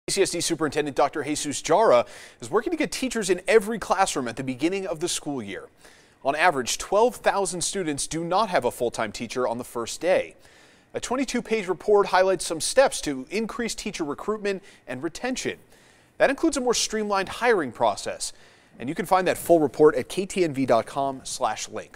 CCSD releases new report about increasing teacher recruitment, retention
Clark County School District (CCSD) Superintendent Dr. Jesus F. Jara has released a new report highlighting possible steps to increase teacher recruitment and retention.